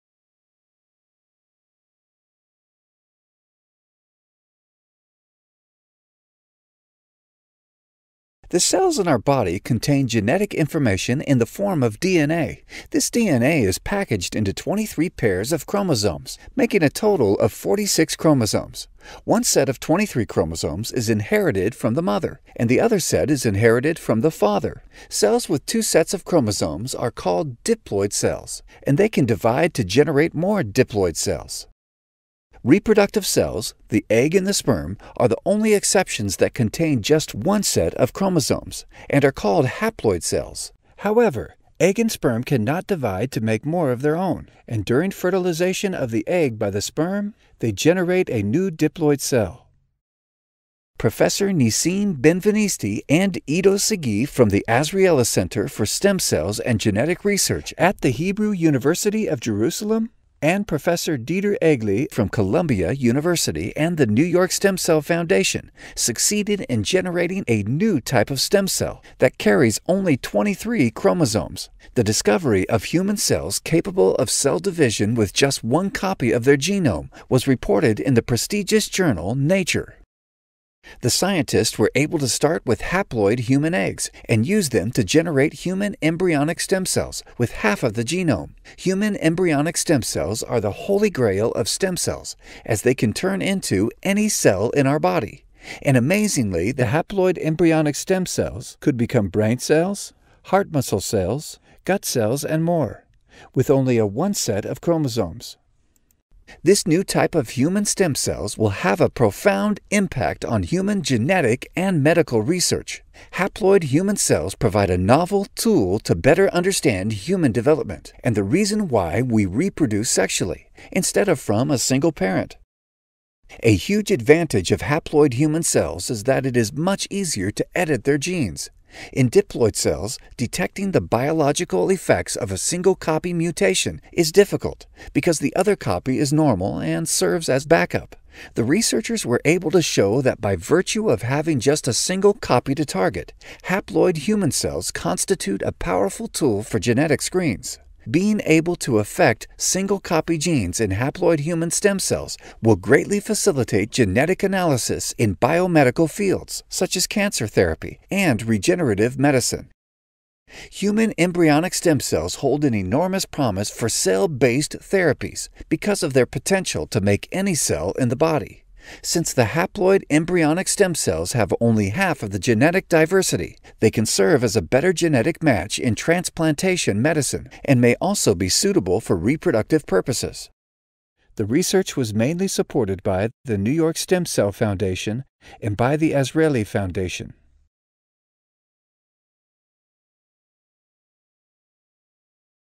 VIDEO: Haploid Human Embryonic Stem Cells
Scientists from The Hebrew University of Jerusalem, Columbia University Medical Center and The New York Stem Cell Foundation Research Institute have succeeded in generating a new type of embryonic stem cell that carries a single copy of the human genome, instead of the two copies typically found in normal stem cells.